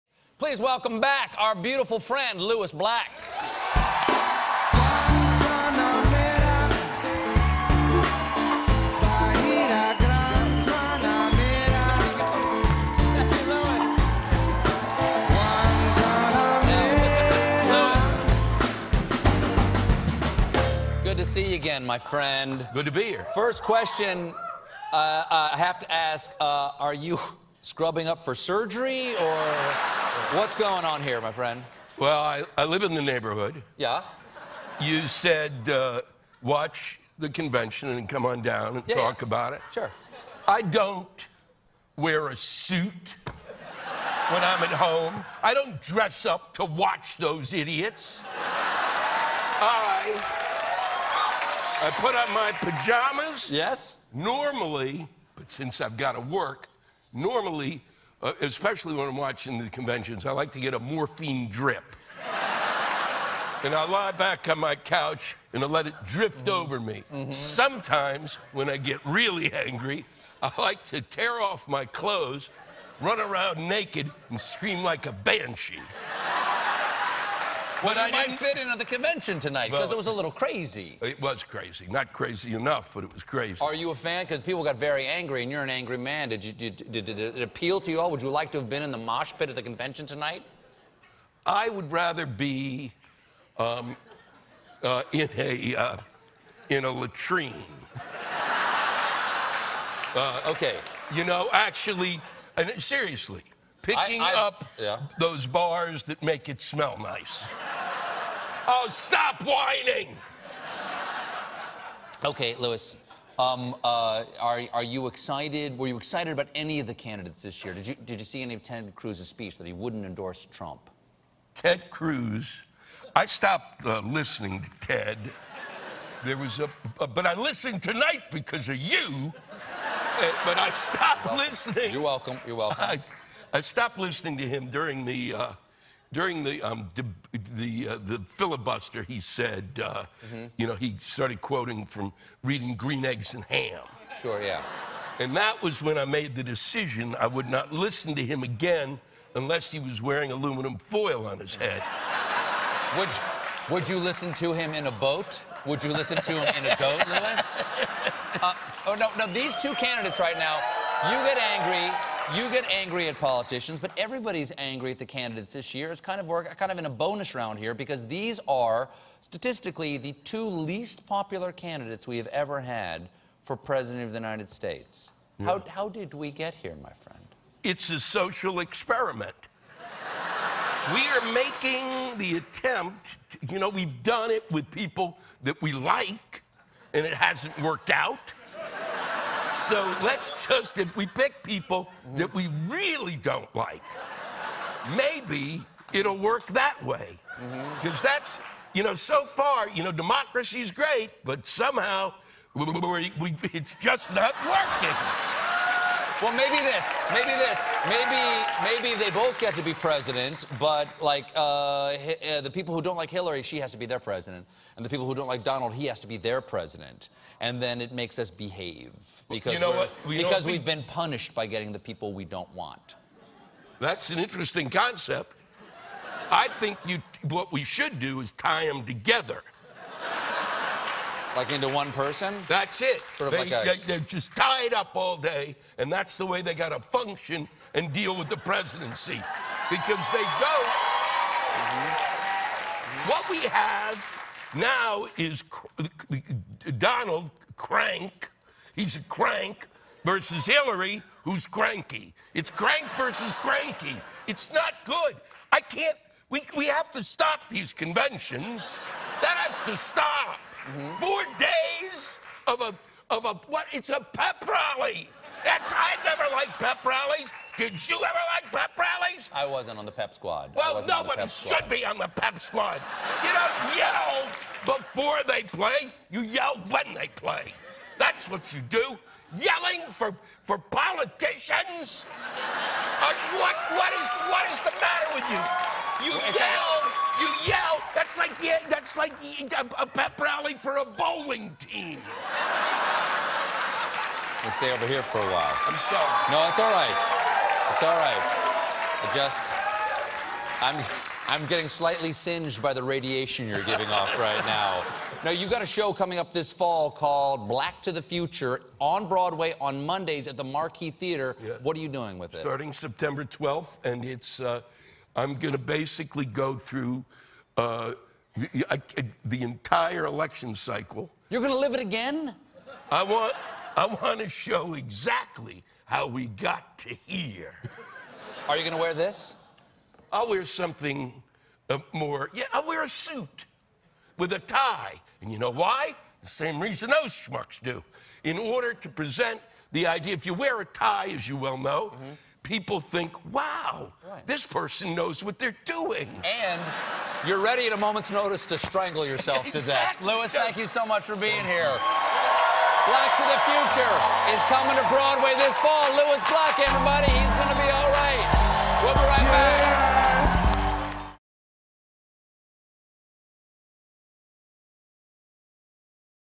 Lewis Black On The Election: "It's A Social Experiment"
The comedian and star of "Back in Black" on Broadway suggests that by choosing between two deeply unpopular presidential candidates, voters are participating in a grand social experiment.